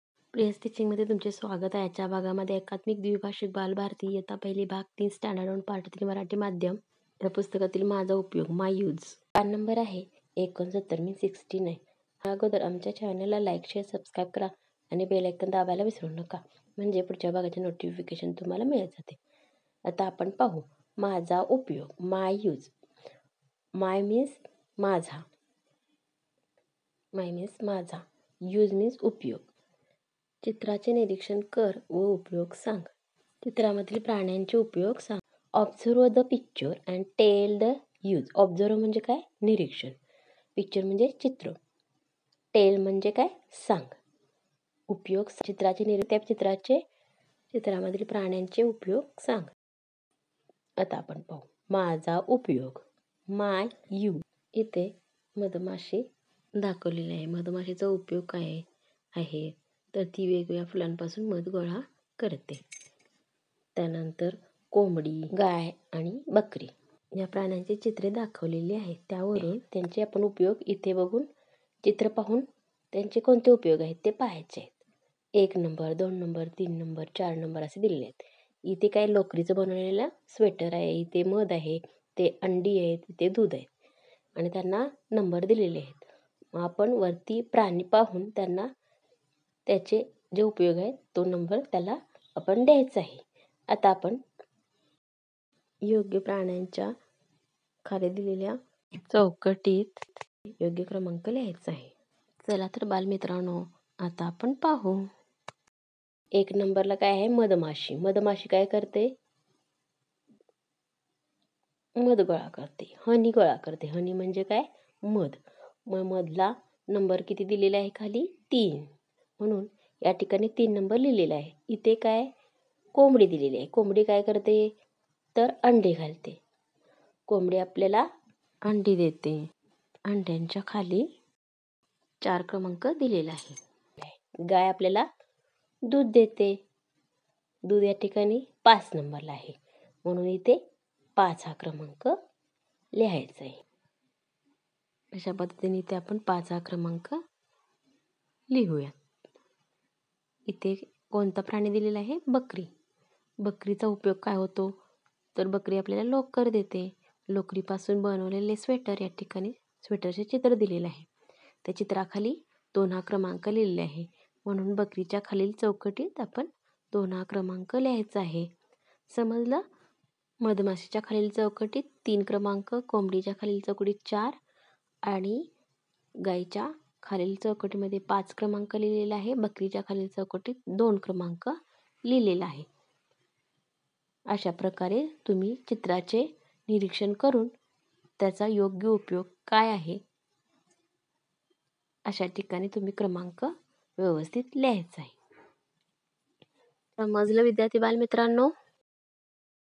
बालभारती, इयत्ता पहिली, भाग -3, पान नंबर- 59, "माझा उपयोग ".
बालभारती, इयत्ता पहिली, भाग -3, पान नंबर- 59,  "माझा उपयोग ". मुलांना  प्राण्यांचे उपयोग समजून सांगणे. प्राण्यांचे उपयोग सांगून  योग्य क्रमांक चित्रा  खाली चौकटीत  लिहिणे